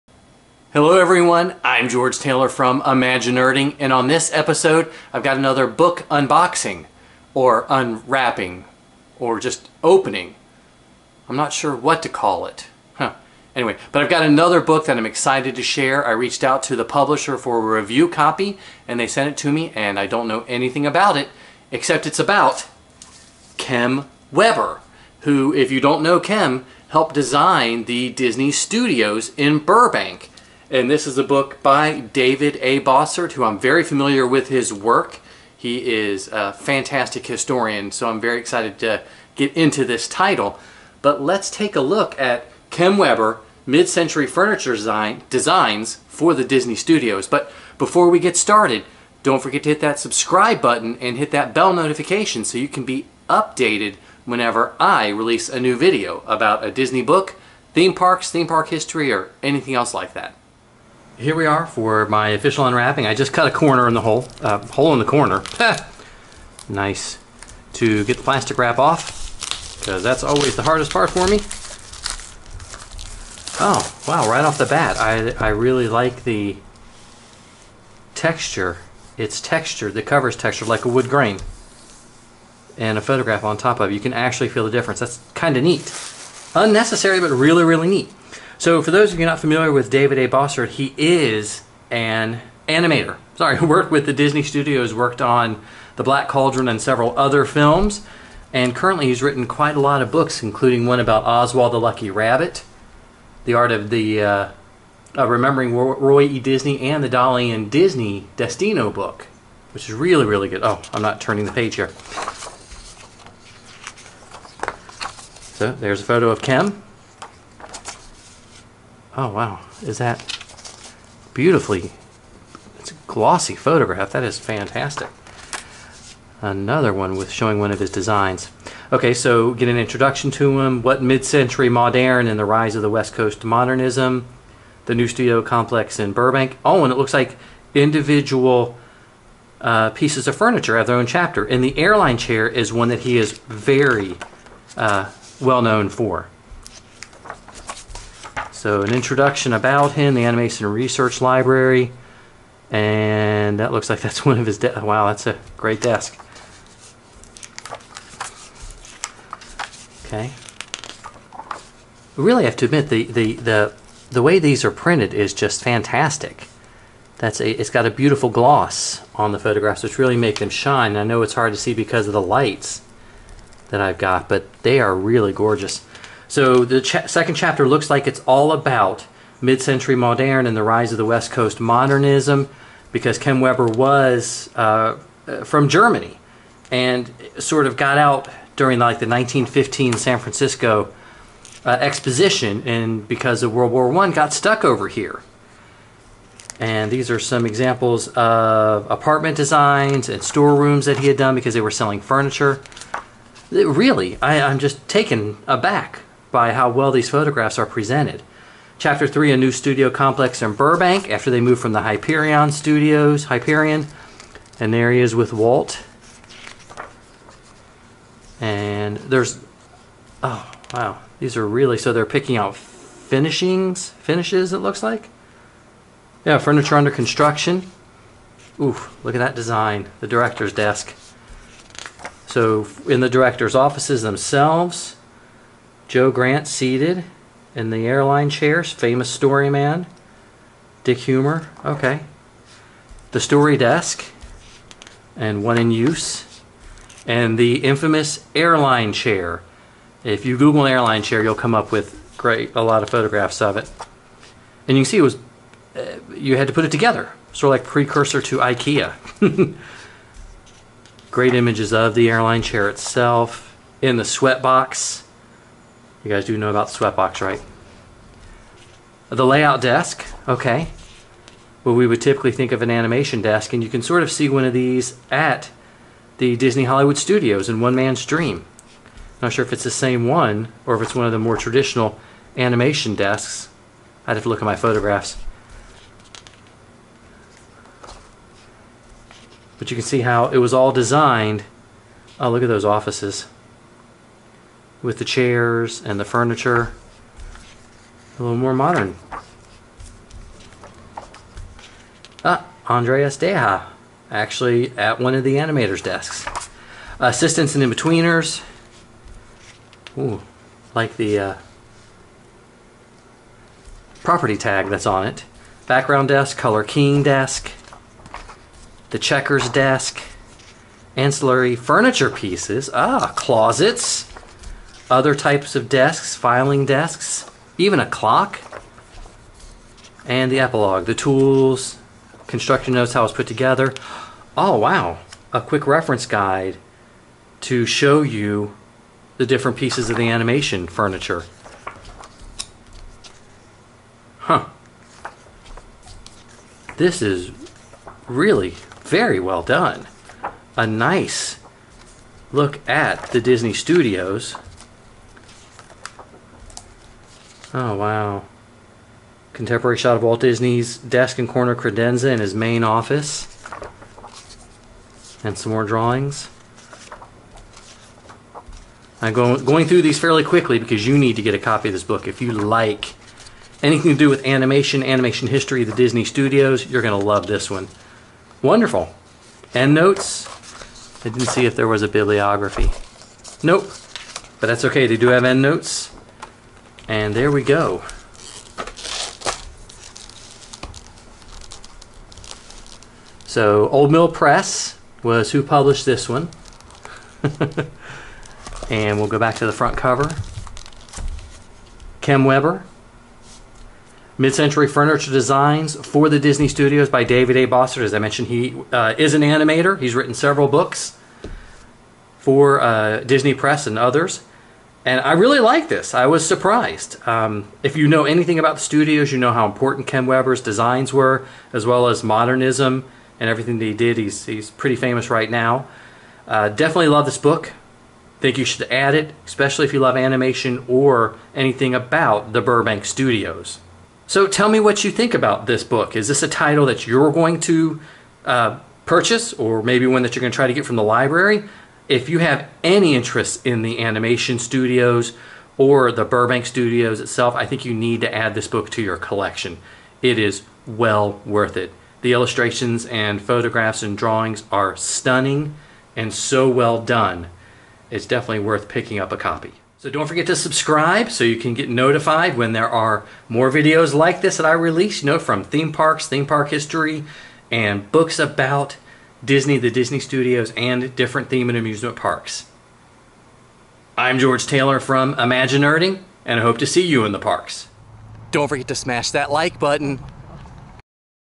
Kem Weber Mid Century Furniture Design for the Disney Studios by David A. Bossert
George gives us a sneak peek of  the new book: Kem Weber Mid Century Furniture Design for the Disney Studios by David A. Bossert.

Kem Weber designed all the buildings, furniture and animation desks for the Disney Studios in Burbank. Kem is famous for his Airplane Chair, which is featured in this book.